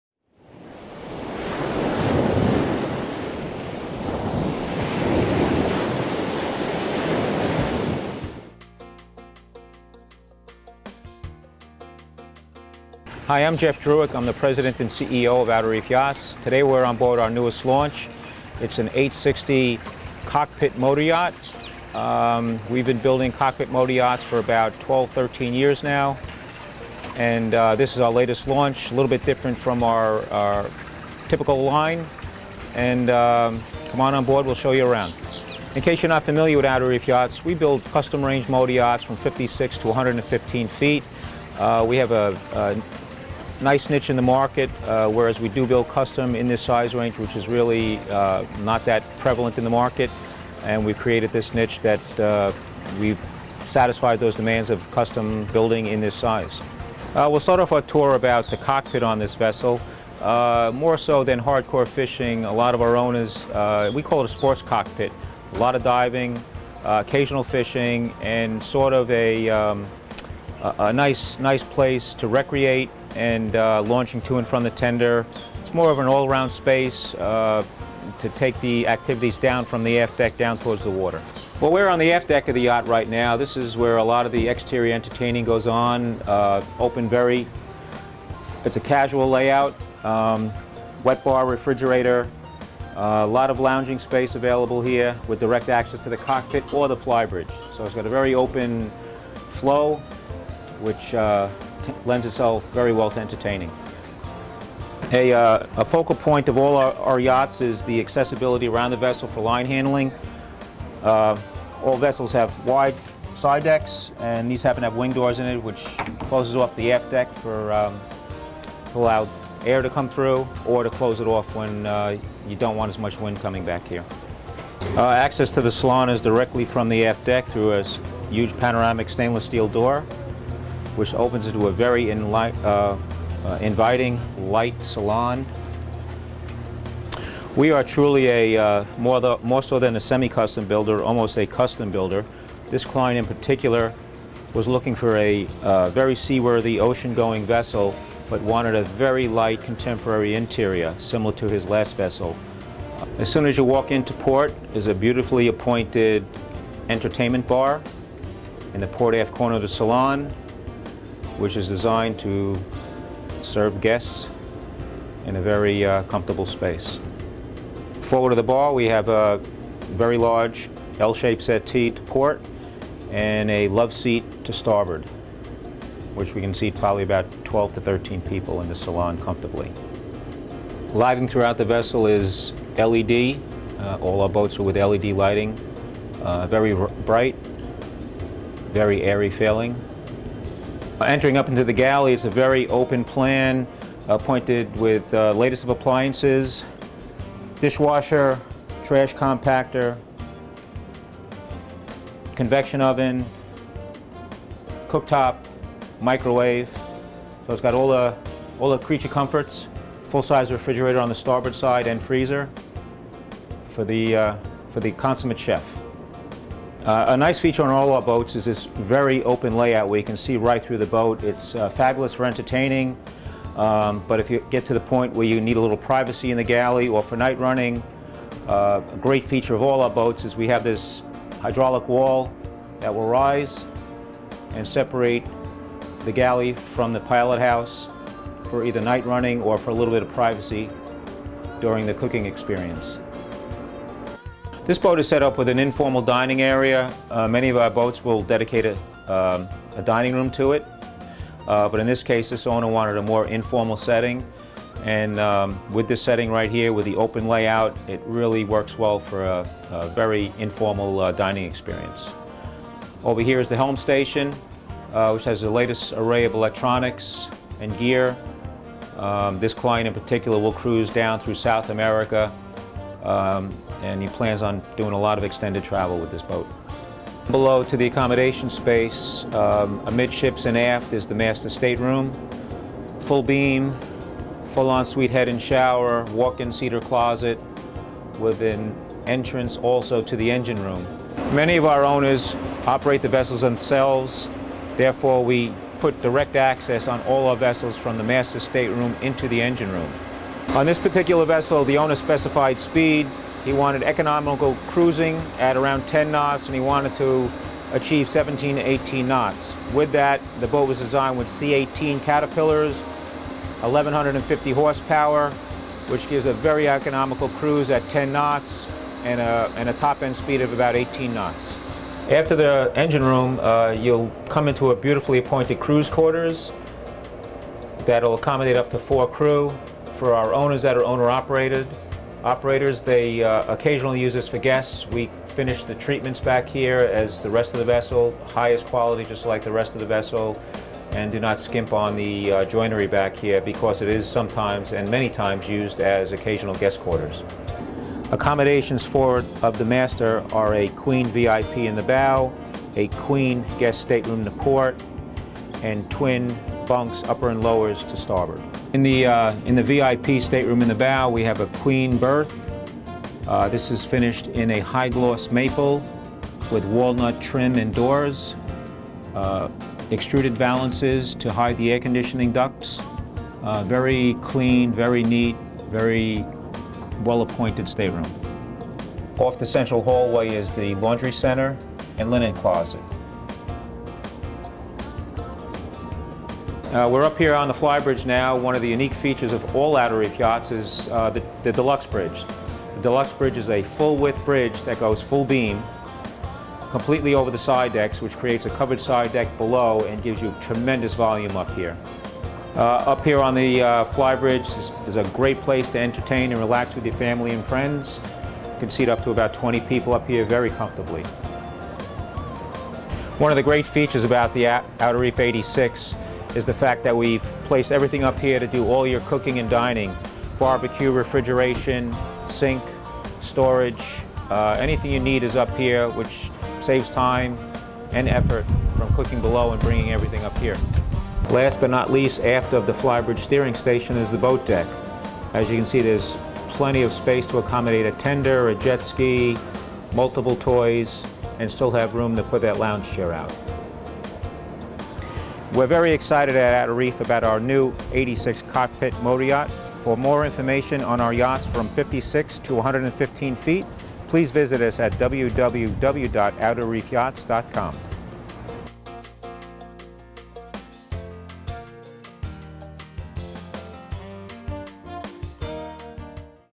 Outer Reef 860 Cockpit Motoryacht at Fort Lauderdale Boat Show
Active Interest Media / PassageMaker Magazine catches up with Jeff Druek, owner of Outer Reef Yachts, at the 2013 Fort Lauderdale International Boat Show to discuss the brand new Outer Reef 860 Cockpit motoryacht